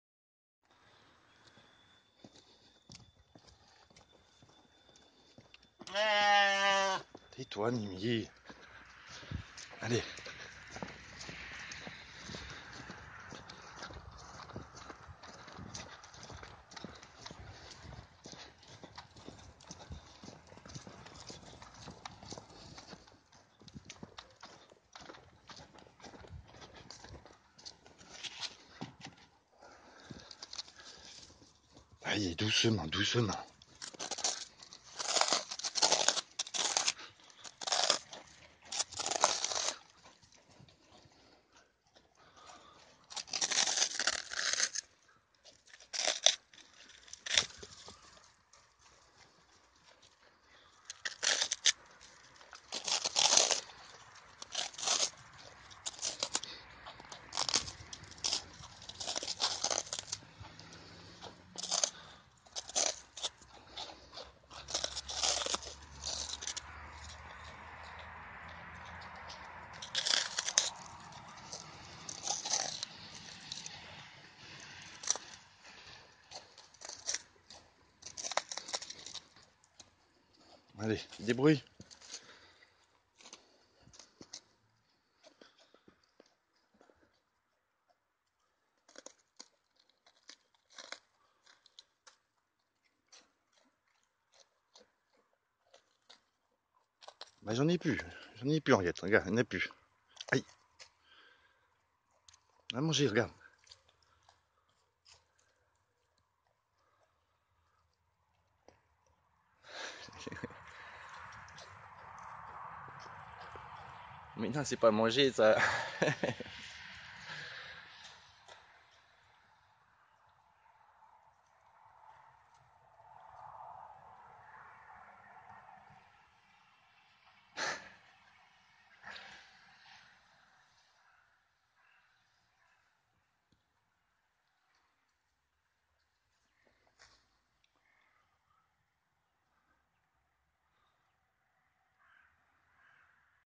Brebis mouton Ouessant et Cameroun
2 brebis d'Ouessant et 2 brebis du Cameroun.